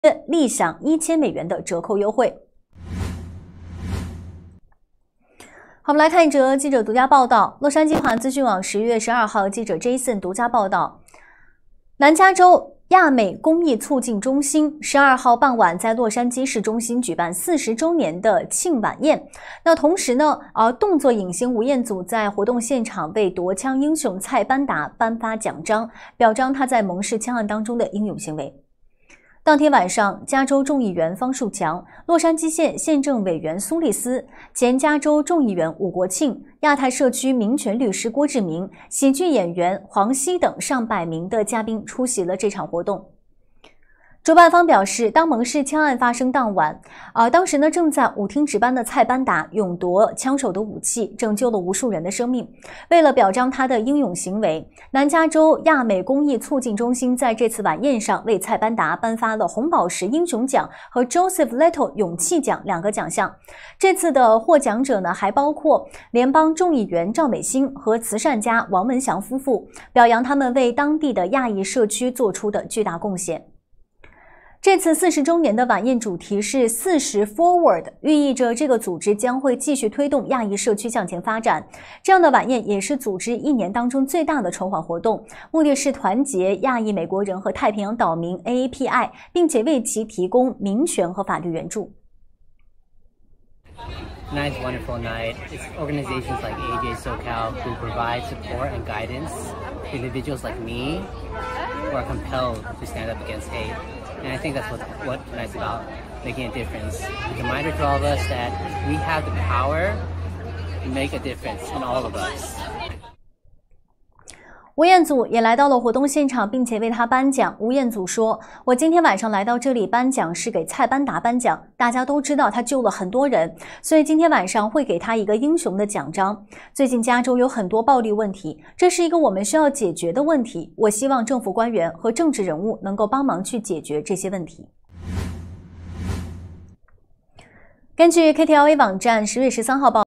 吴彦祖现身为蒙市枪案夺枪英雄蔡班达颁奖 20231013
最后一波 ，政府奖励金，企业主加他： 全美权威20年经验会计事务所，Jack Lin专业erc申请5000+成功案例。免费咨询 不成功不收费 .